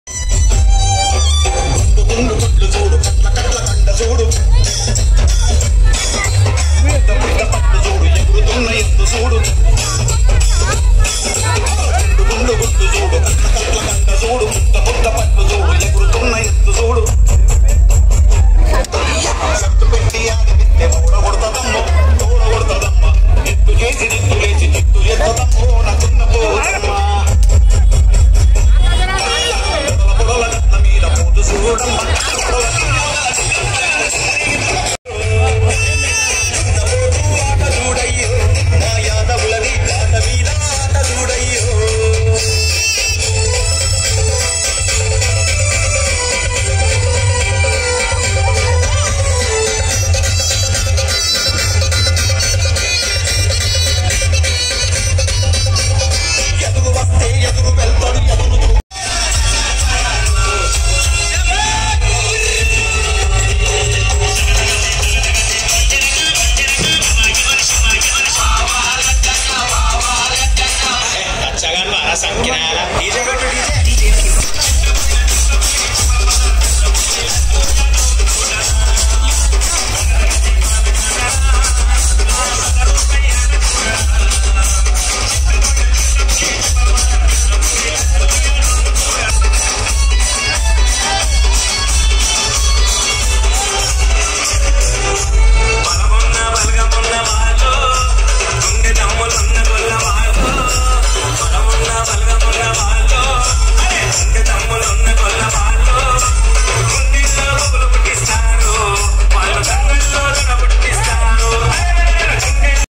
Swapna yadav dance with her father | Sadar festival 2022
Shivarampally swapna yadav sadar 2022 | swapna yadav dance with her father in sadar festival. @swapnayadav_official2001